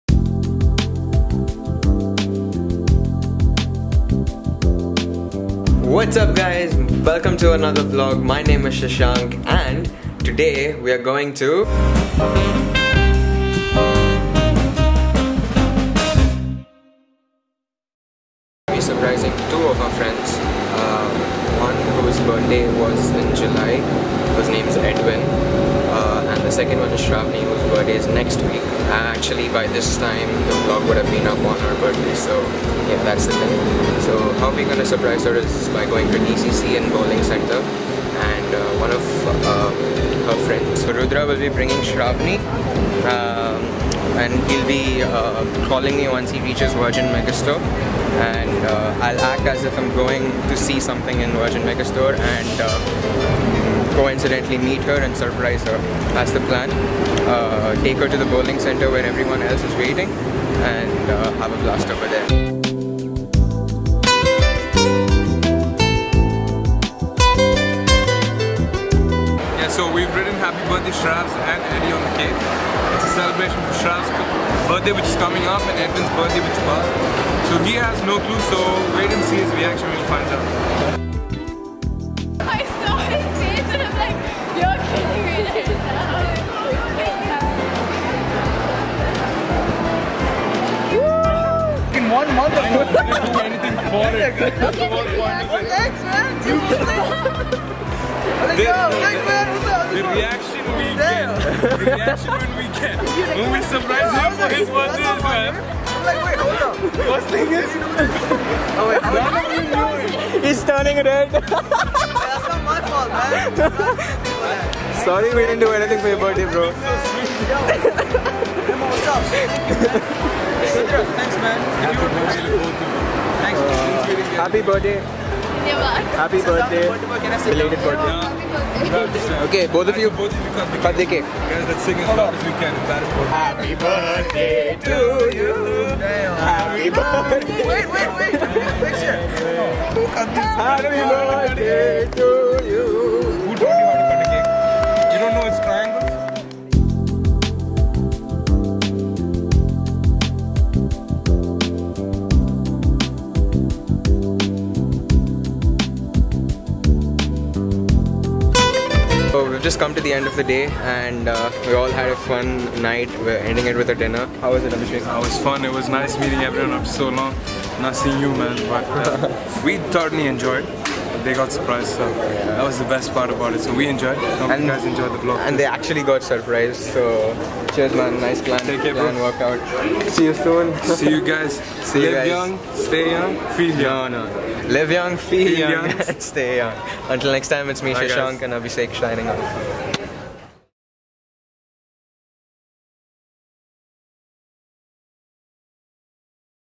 SURPRISING TWO OF OUR FRIENDS | VlogWithShashank
Surprising two of our friends, Edwin who's birthday was last month and Shravani who's birthday is today were surprised by bunch of six friends in Deira City Centre here in Dubai.

This video was filmed on Canon Eos 750D & The Samsung Galaxy S6 Edge.